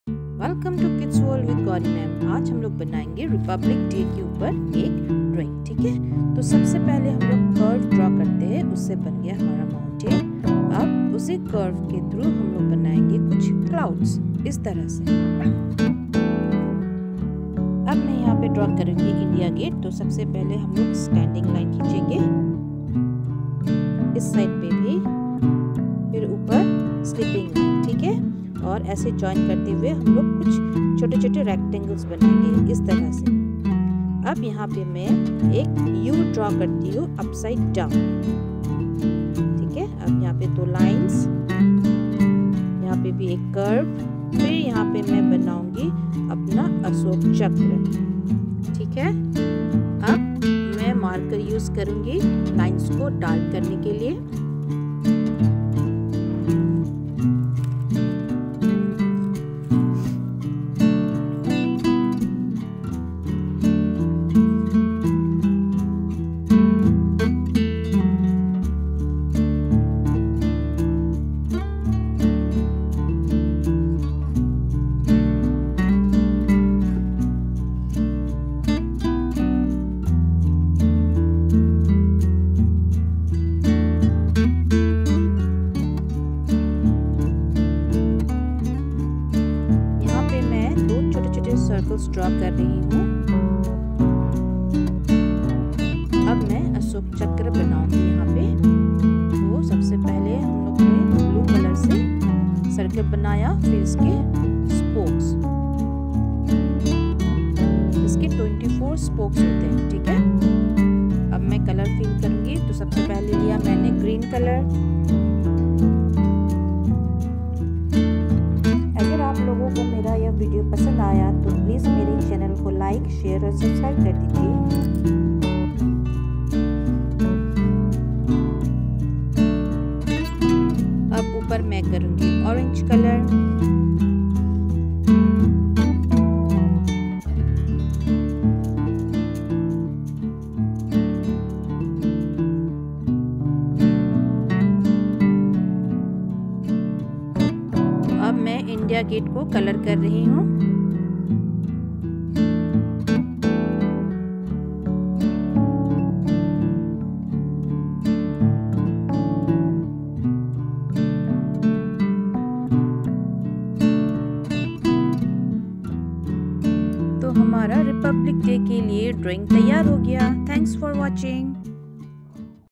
Republic day Drawing || 26 Jan Drawing
Hello! Kids,

In this video, child will learn how to draw Republic day Drawing.